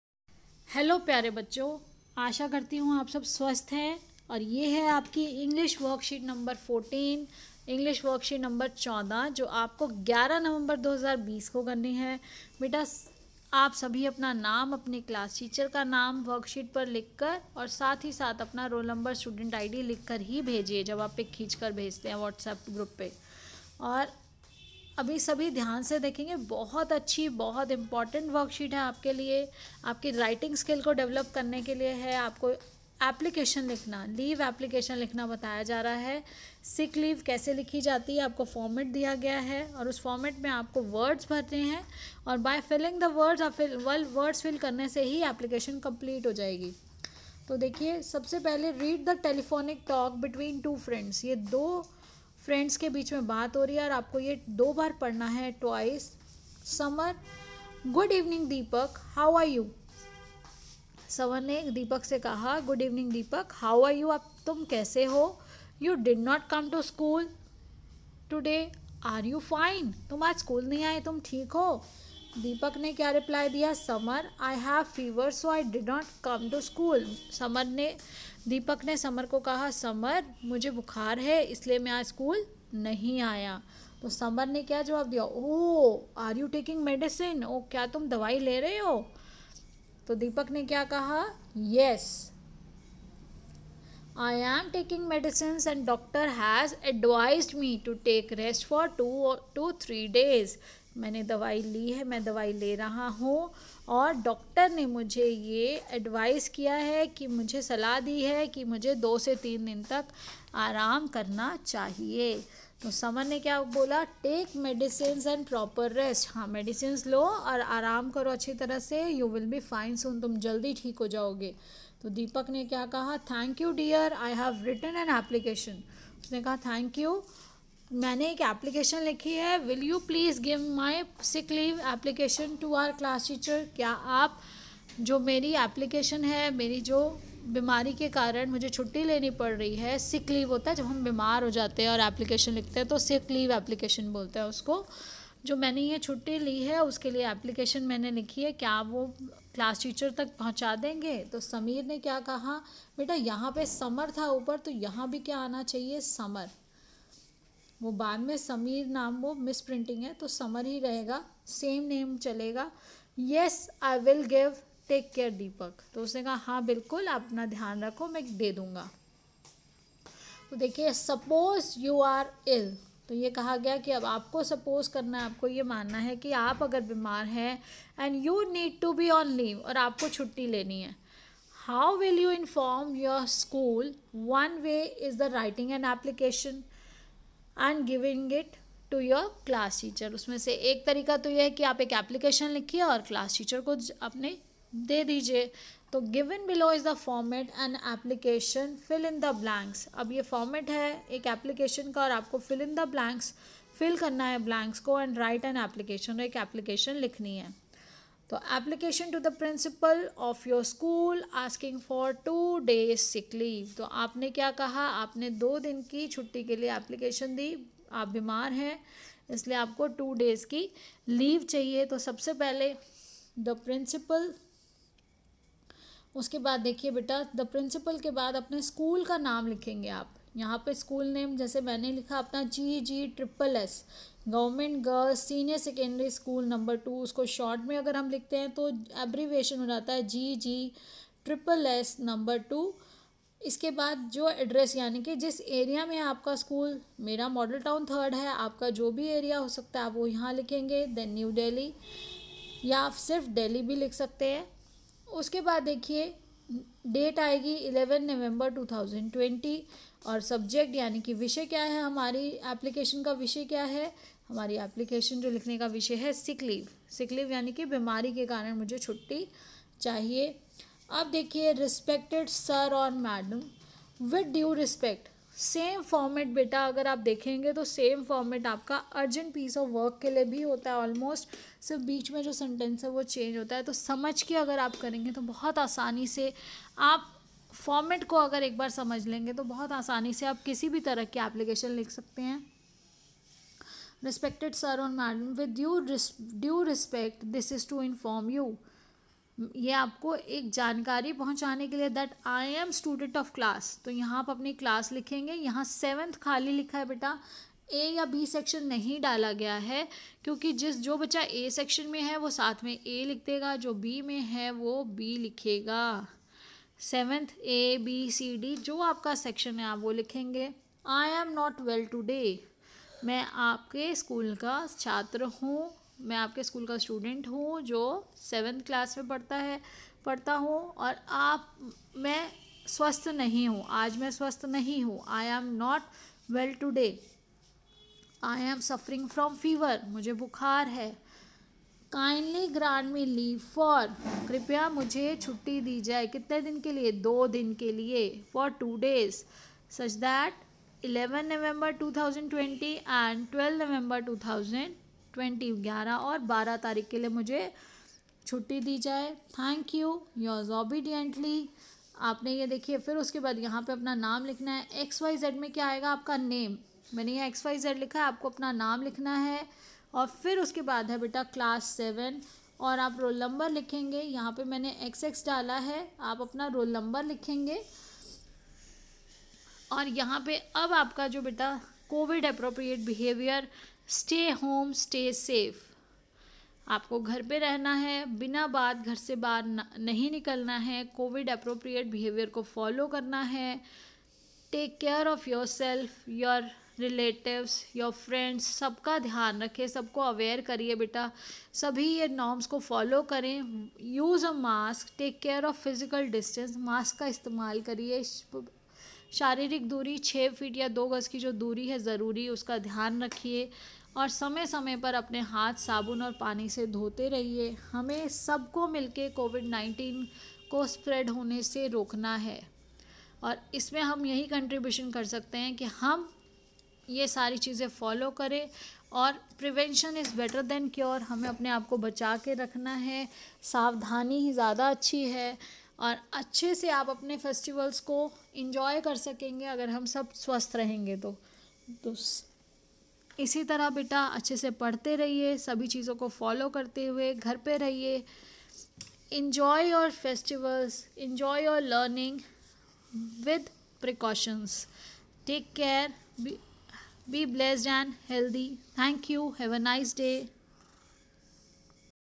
CLASS 7 ENGLISH WORKSHEET 19|23-9-2021|APPLICATION FOR SICK LEAVE |EXPLORE ENJOY AND ENRICH
simple  conversation and format with solution for sick leave